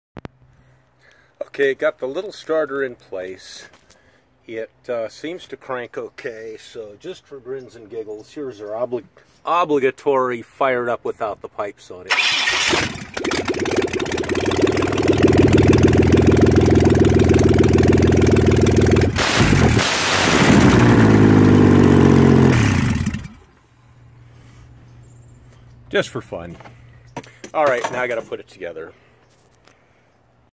First motor - '87 3.8 in Toy. Changed starter, quick startup without exhaust manifold
making noise, fire from ports